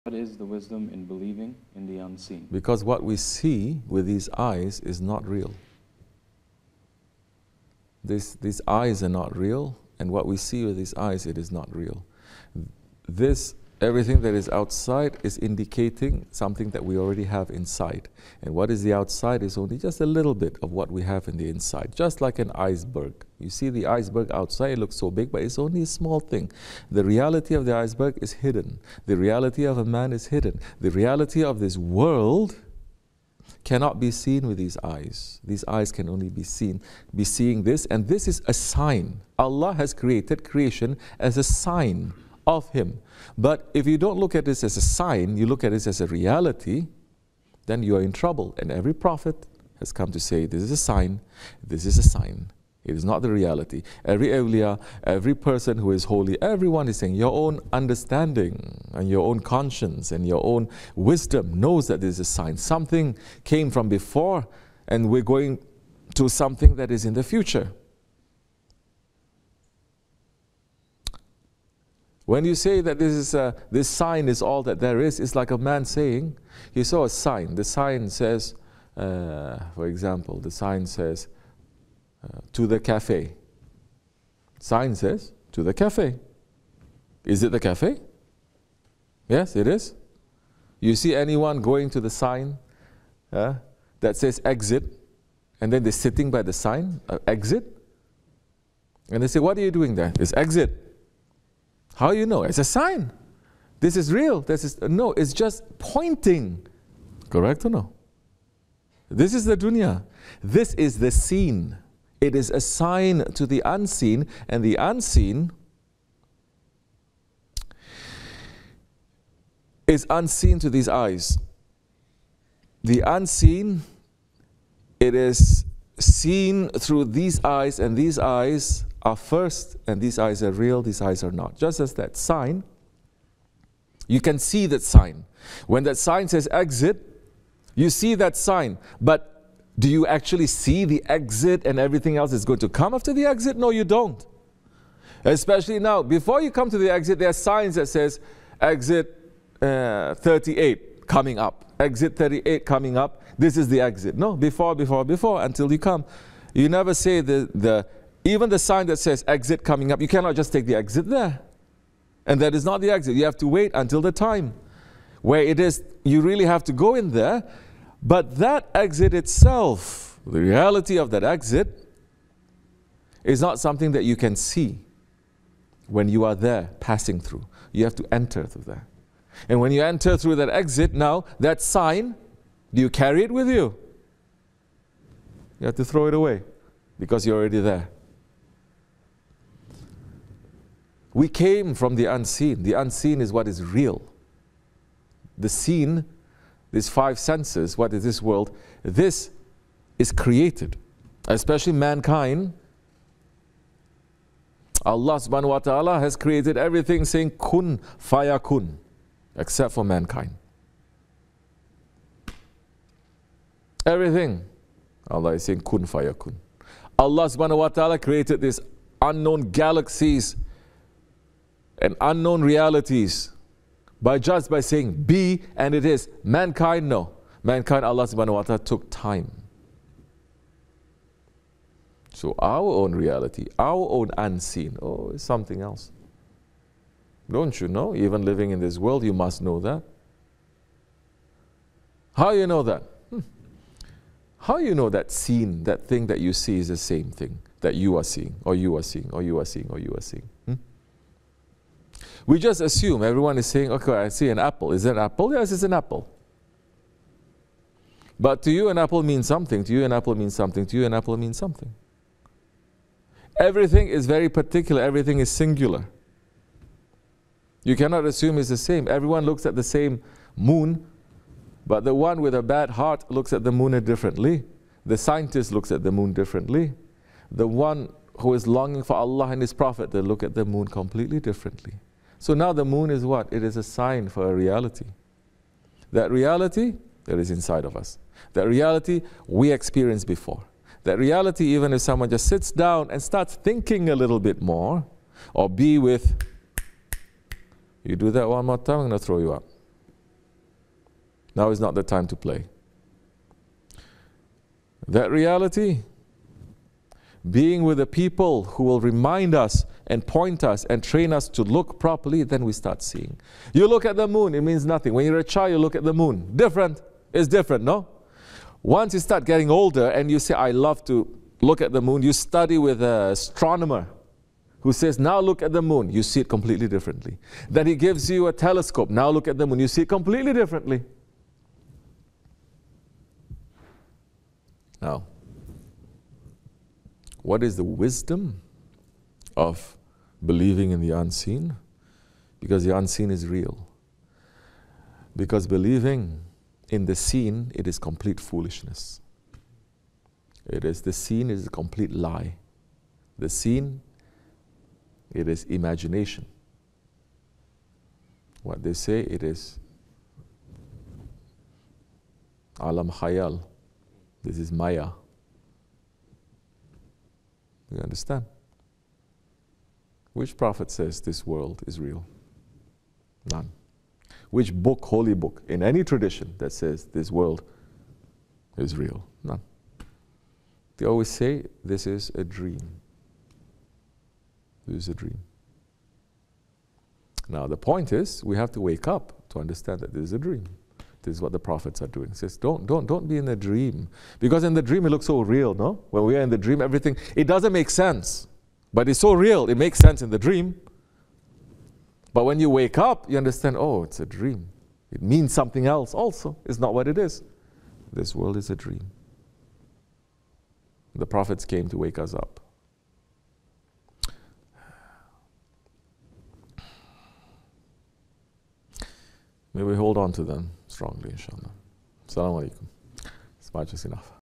What is the wisdom in believing in the unseen?
Shaykh Lokman Efendi Hz
Waris of SahibulSaif Shaykh Abdulkerim el Kibrisi (qs),
Osmanli Dergahi, Sidney Center, NY
5 Safar 1441H

U.S.A.